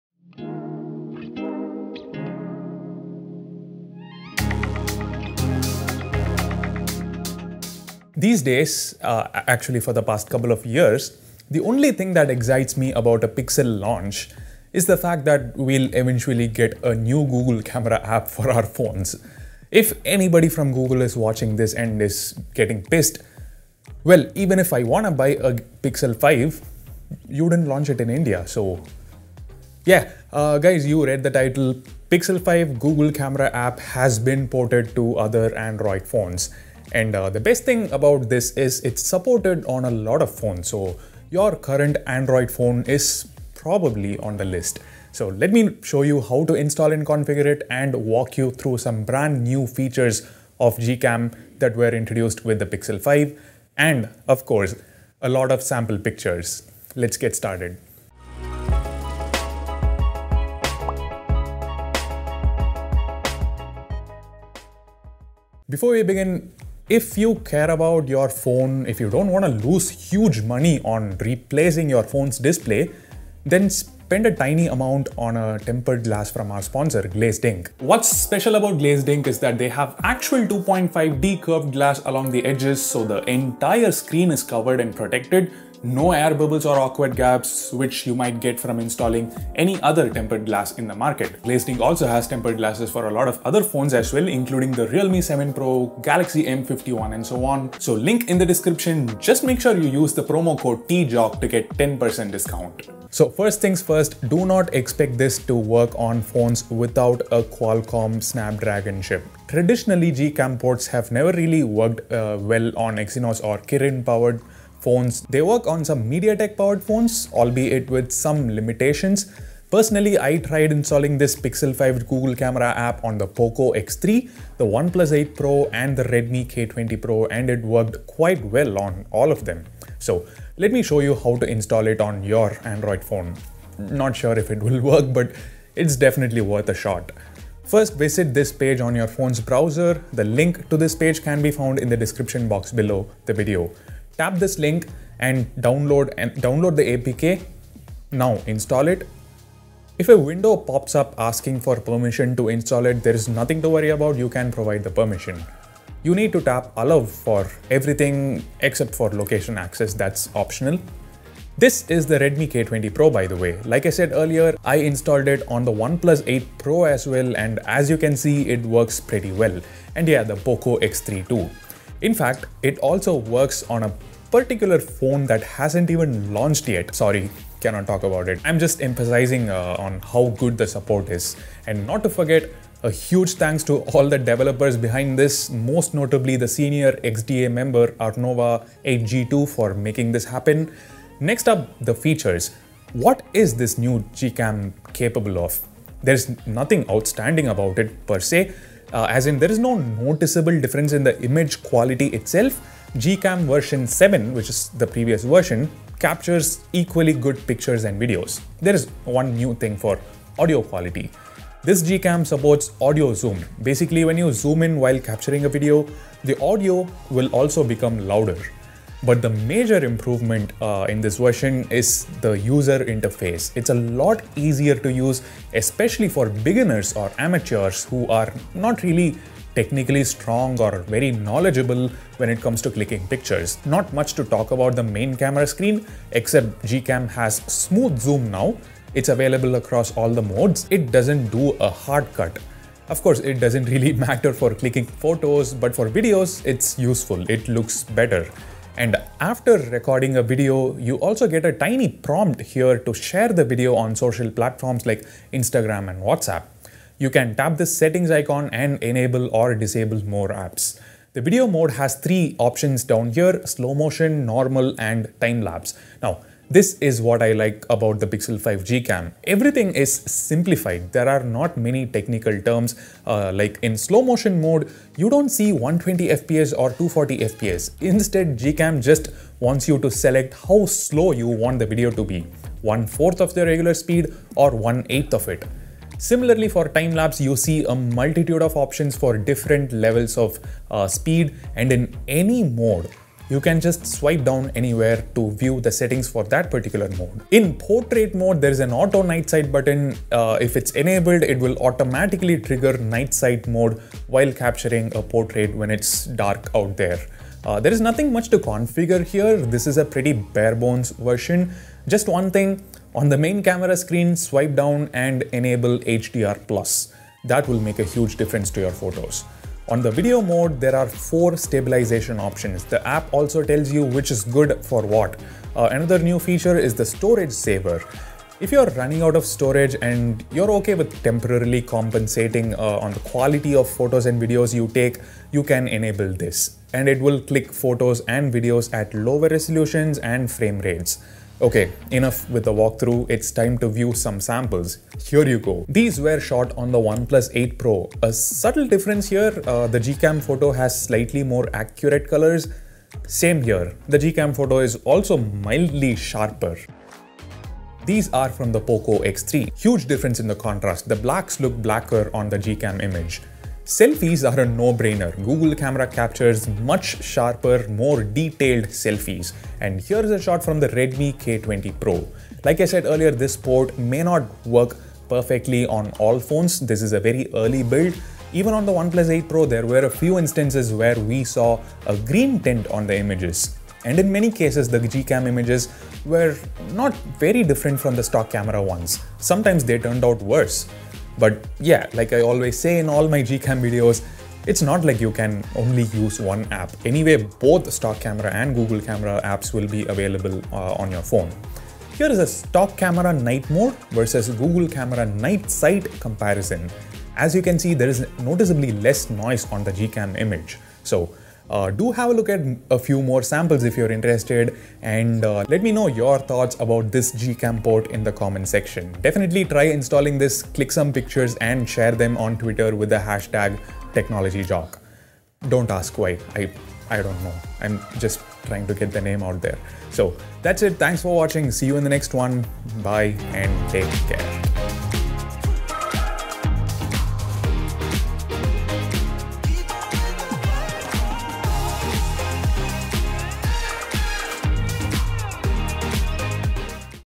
Pixel 5 GOOGLE CAMERA Port for Other Android Phones (NO ROOT) + GCam Samples
Pixel 5 GCam for OnePlus 8T, Redmi K20 Pro, Poco X3, and more Android Phones!

► DOWNLOADS

Thanks to the senior XDA developer Arnova8G2 for making this happen!

Today's Best Amazon Deals
Today's Best Flipkart Deals
Topics:

00:00 - Intro
01:02 - Sponsor
01:43 - Compatibility
02:17 - How To Install
03:10 - New Features
05:32 - GCam vs Stock Cam Samples from 3 Phones
06:54 - Conclusion

Share on Facebook, Twitter, WhatsApp - wherever possible!